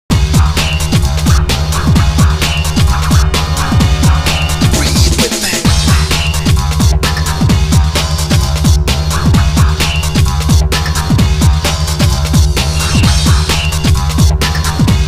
120cm Salamander dinner time
Say "Hallo" to "Karlo" 🦎

Chinese Giant Salamander(Andrias davidianus)

At the @naturkundemuseumkarlsruhe
Personal caregiver.@vivaristikjunkie